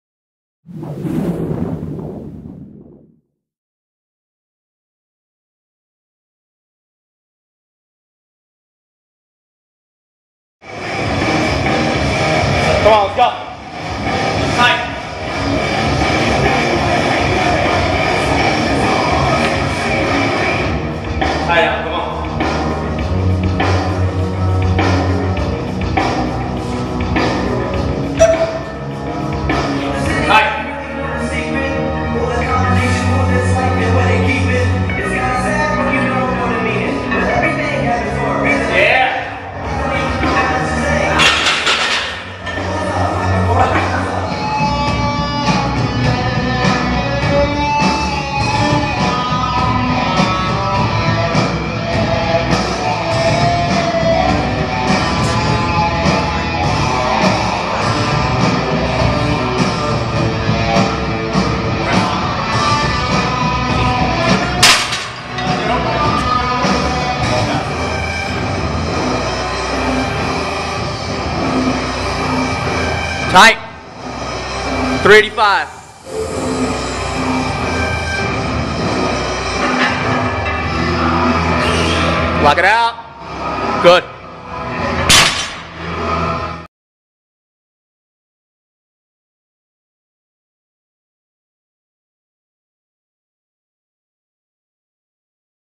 Ellen Stein (61 years young) Squat 295, Bench 165, Deadlift 385
3/25/14
132 lb. weight class...
All Iron Arena gym records, all effortless!